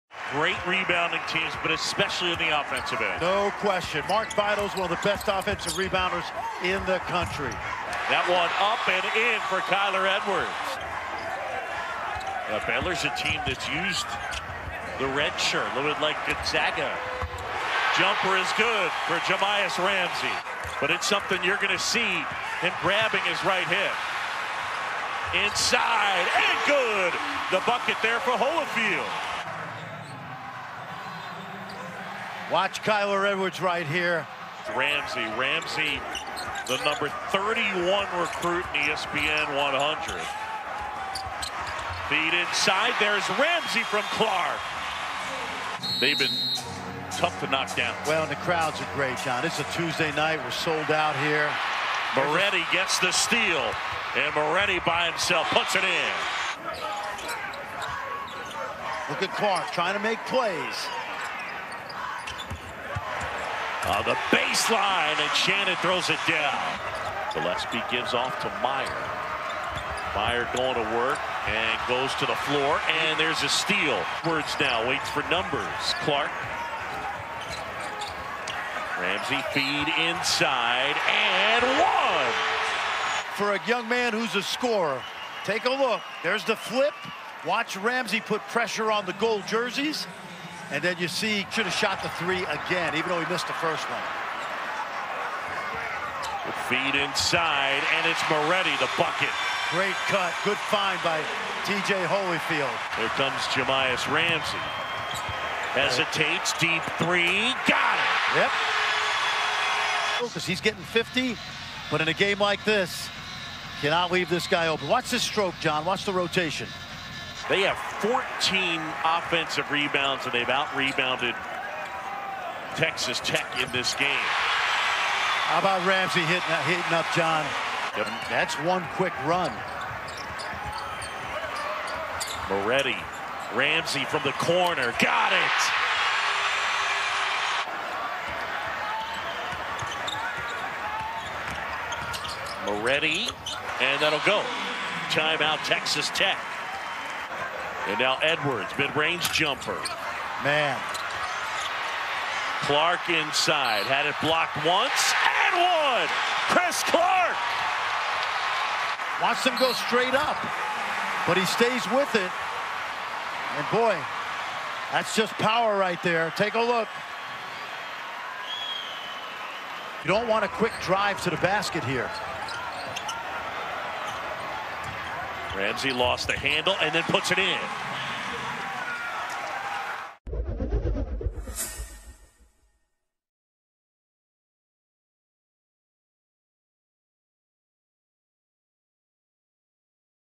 No. 22 Texas Tech Basketball Top Plays vs. No. 4 Baylor (2019-20) | Stadium
Texas Tech G Jahmi'us Ramsey put in 20 points and grabbed five rebounds, and G Kyler Edwards added 10 points in the Red Raiders' 57-52 loss on Tuesday. Check out the top plays from TTU in their tough loss here.